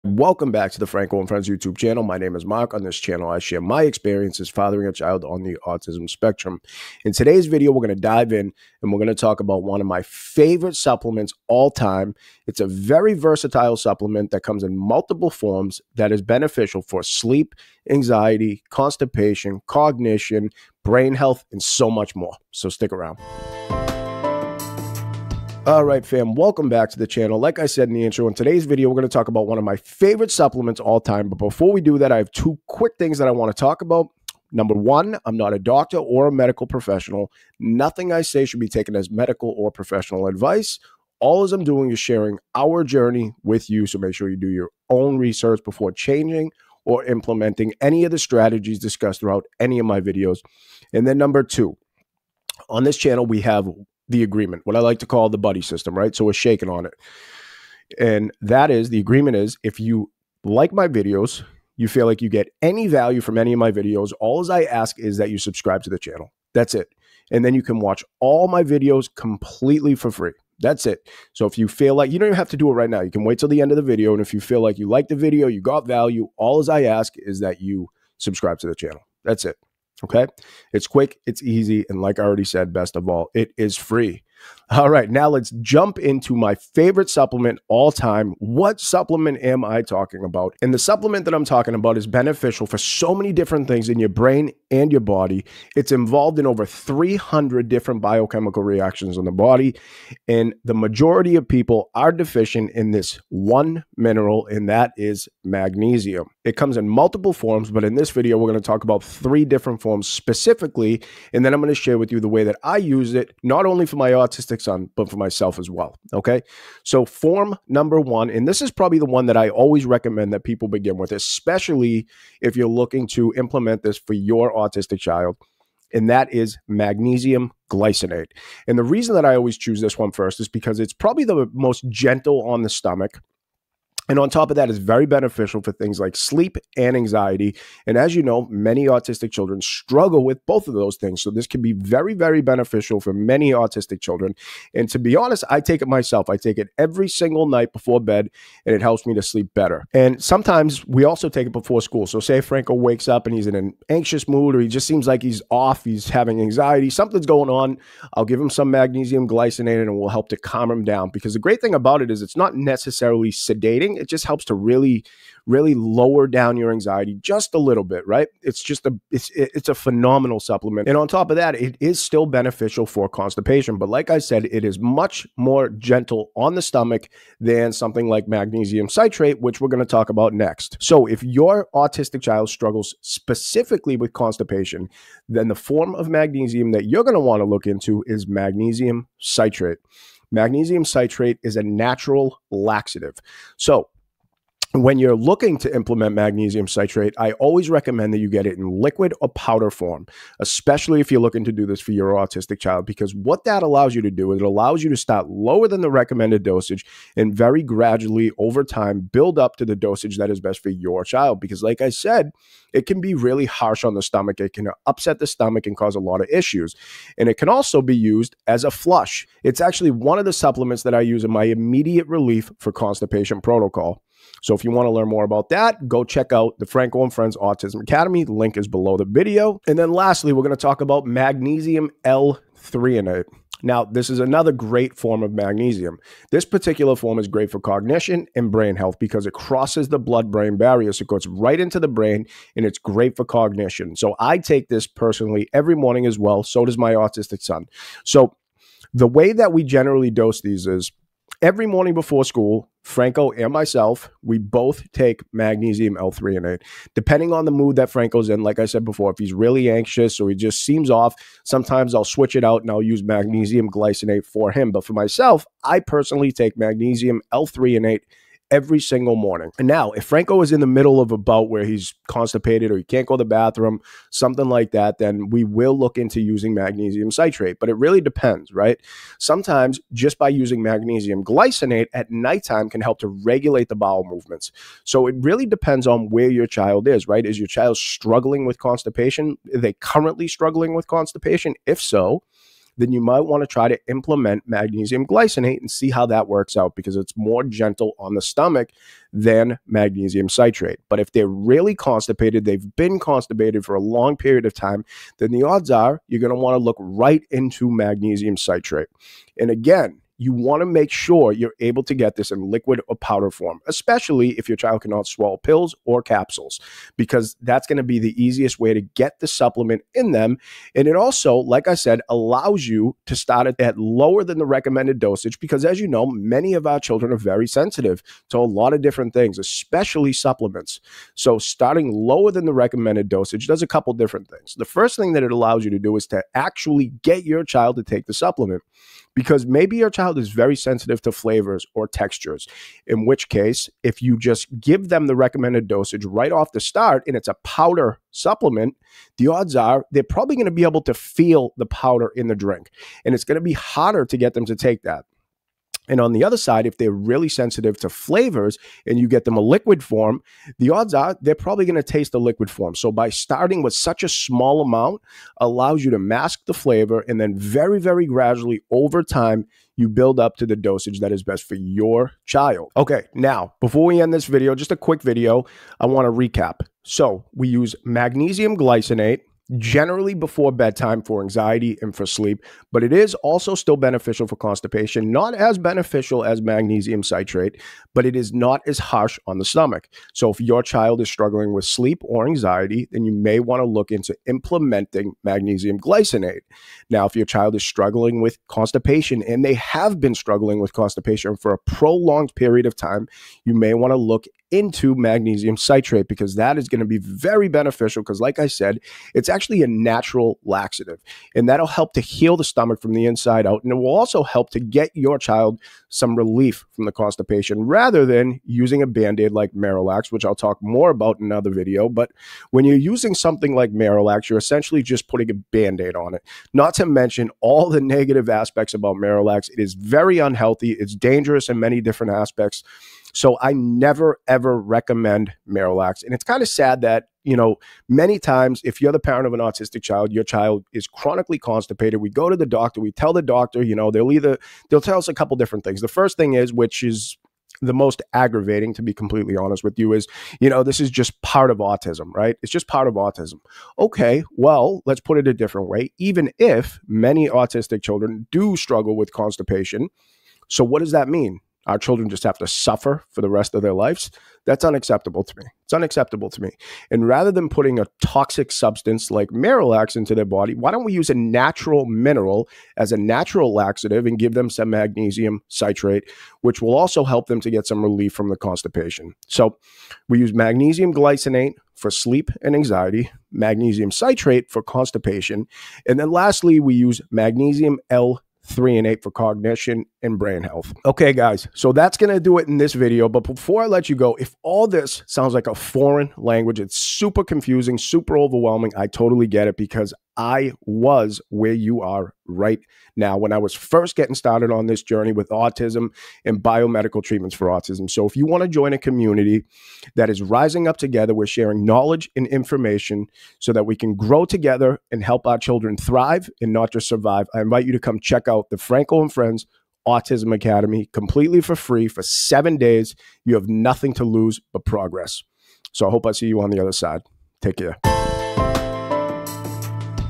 Magnesium A GAME CHANGER For AUTISM? What Every Parent Should Know BEFORE Starting Magnesium
In today's video, we answer the question is magnesium a game changing supplement for autism. We also discuss the three best forms of magnesium and the best ways to use each.